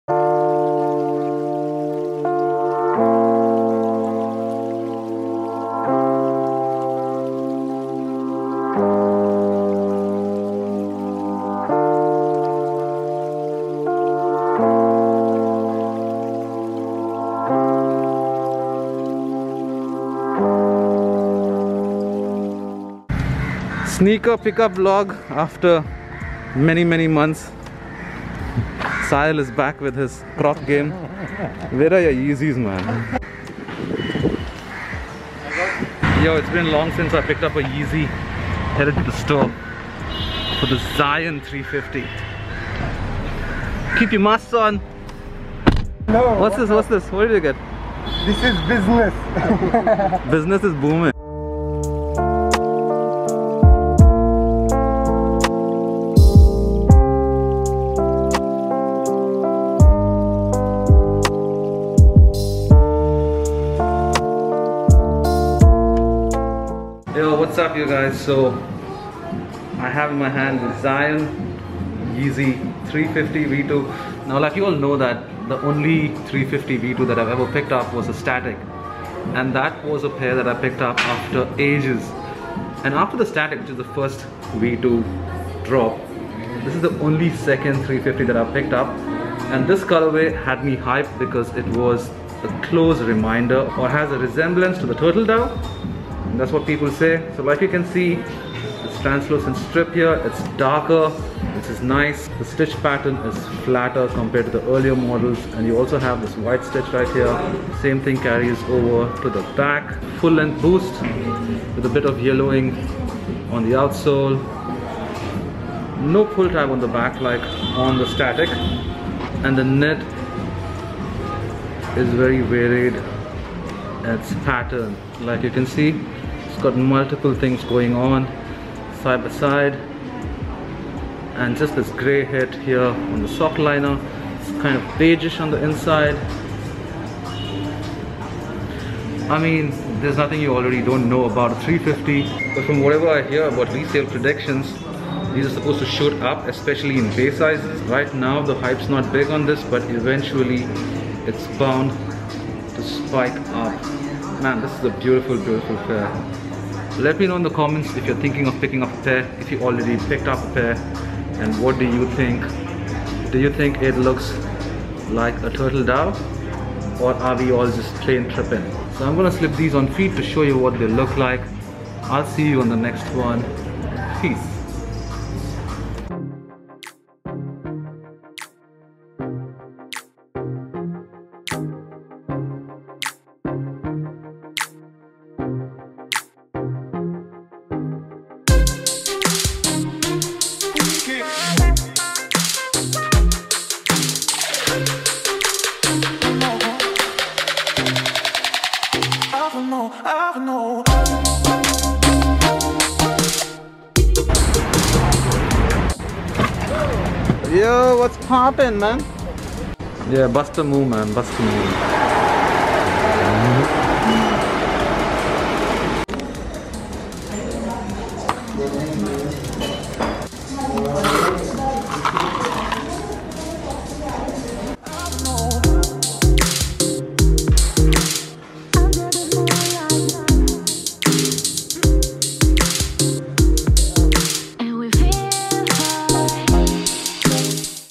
Yeezy Boost 350 Zyon PickUp And On Feet Review + Daily Vlog Tings
After a long long long time, did a Yeezy 350 excite me. The Zyon got the market excited because of it's (some people think, including me) resemblance to the OG Turtle Dove. I had to go and find out for myself. What are your thoughts?

Especially given that we are under lockdown, I hope you all are staying safe. Make sure to leave a comment please, and do like the video if the content was something that works for you.

Don't forget to tell a friend to tell a friend.

––– ♪♫ Artists' Links ♫♪ –––